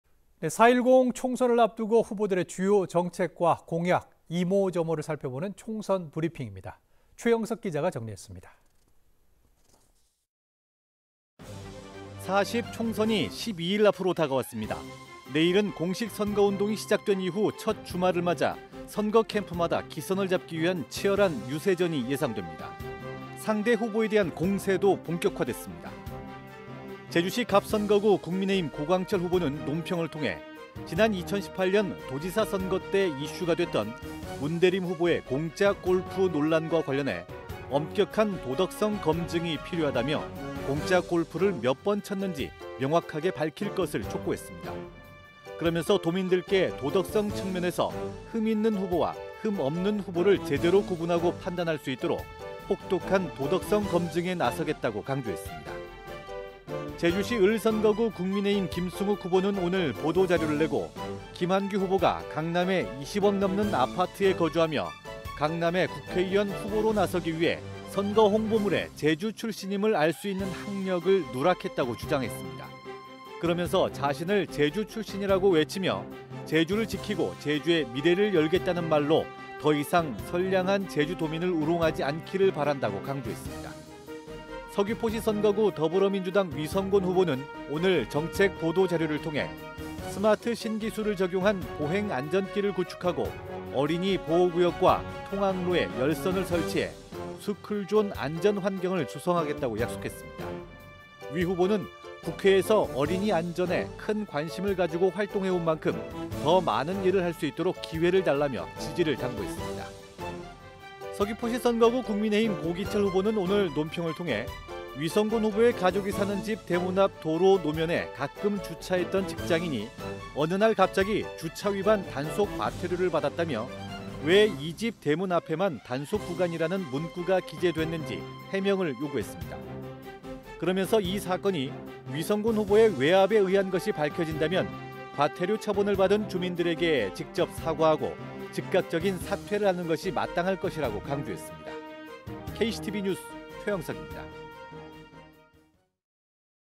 총선 브리핑 D-12
4.10 총선을 앞두고 후보들의 주요 정책과 공약, 이모저모를 살펴보는 총선 브리핑입니다.

최형석 기자가 정리했습니다.

4.10 총선이 12일 앞으로 다가왔습니다.

내일은 공식 선거운동이 시작된 이후 첫 주말을 맞아 선거 캠프마다 기선을 잡기 위한 치열한 유세전이 예상됩니다.

상대 후보에 대한 공세도 본격화됐습니다.

제주시 갑 선거구 국민의힘 고광철 후보는 논평을 통해 지난 2018년 도지사 선거때 이슈가 됐던 문대림 후보의 공짜 골프 논란과 관련해 엄격한 도덕성 검증이 필요하다며 공짜 골프 몇 번 쳤는지 명확하게 밝힐 것을 촉구했습니다.

그러면서 도민들께 도덕성 측면에서 흠 있는 후보와 흠 없는 후보를 제대로 구분하고 판단할 수 있도록 혹독한 도덕성 검증에 나서겠다고 강조했습니다.

제주시 을 선거구 국민의힘 김승욱 후보는 오늘 보도자료를 내고 김한규 후보가 강남에 20억 넘는 아파트에 거주하며 강남의 국회의원 후보로 나서기 위해 선거 홍보물에 제주 출신임을 알 수 있는 학력을 누락했다고 주장했습니다.

그러면서 자신을 제주 출신이라고 외치며 제주를 지키고 제주의 미래를 열겠다는 말로 더이상 선량한 제주도민을 우롱하지 않기를 바란다고 강조했습니다.

서귀포시 선거구 더불어민주당 위성곤 후보는 오늘 정책 보도자료를 통해 스마트 신기술을 적용한 보행 안전길을 구축하고, 어린이 보호구역과 통학로에 열선을 설치해 스쿨존 안전환경을
조성하겠다고 약속했습니다.

위 후보는 국회에서 어린이 안전에 큰 관심을 가지고 활동해 온 만큼 더 많은 일을 할 수 있도록 기회를 달라며 지지를 당부했습니다.

서귀포시 선거구 국민의힘 고기철 후보는 오늘 논평을 통해 위성곤 후보의 가족이 사는 집 대문앞 도로노면에 가끔 주차했던 직장인이 어느날 갑자기 주차위반 단속과태료를 받았다며 왜 이 집 대문앞에만 단속구간이라는 문구가 기재됐는지 해명을 요구했습니다.

그러면서 이 사건이 위성곤 후보의 외압에 의한 것이 밝혀진다면 과태료 처분을 받은 주민들에게 직접 사과하고 즉각적인 사퇴를 하는 것이 마땅할 것이라고 강조했습니다.

KCTV뉴스 최형석입니다.

(영상편집 박병준, 그래픽 소기훈)